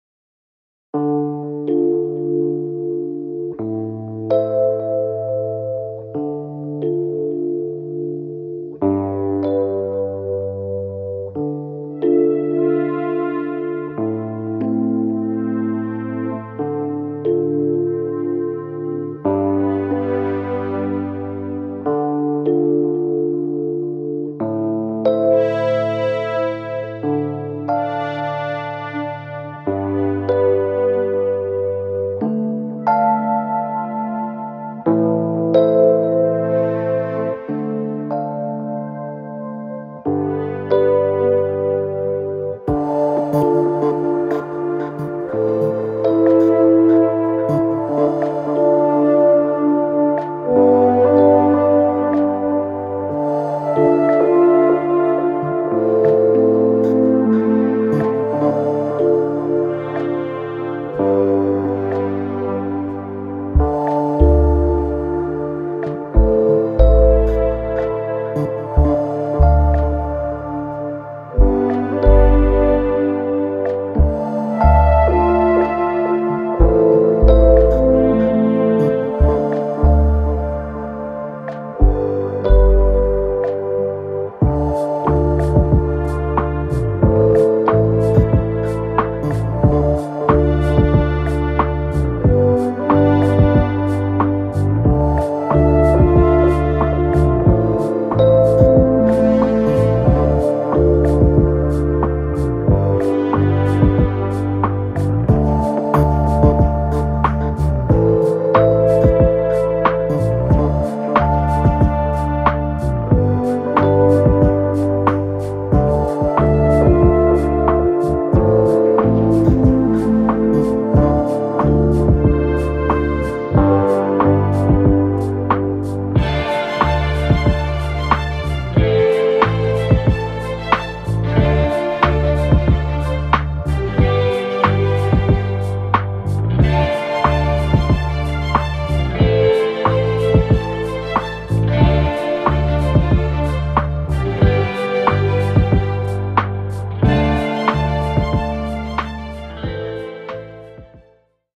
Paragliding: Horné Orešany / Slepý vrch - Smolenice (End of summer 2021) [4K]
A short trip from Slepý Vrch to Smolenice Castle.